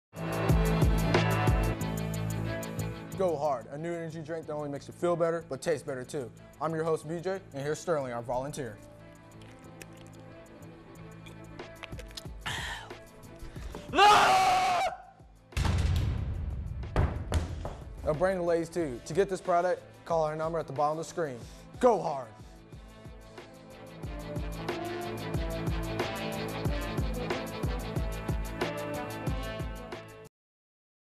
Go Hard
This is a 30 second commercial that was made in Greg Ruuska's Studio 1 class in the Spring of 2007 at North Lake College.